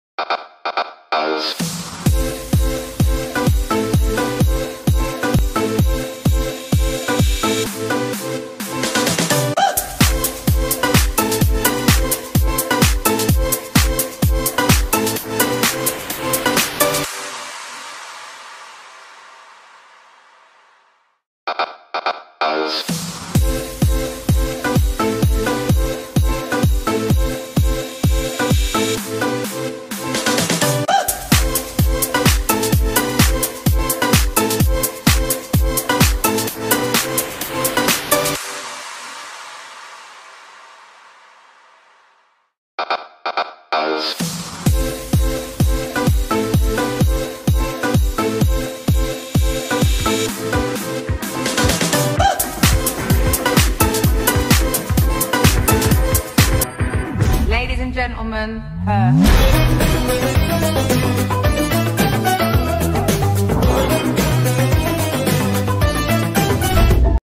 50 gacha outfits that are not in the game
At the exception of 36 and 45, those outfits cannot be created in the game. However, I am the one who drew every single one of those, and the mannequin. Inspire yourself if you want, just don’t copy those unless you give me credit!

For the “ladies and gentlemen… her” sound, credit to @quitezyaudios .

Remember: anyone can wear these, male or female.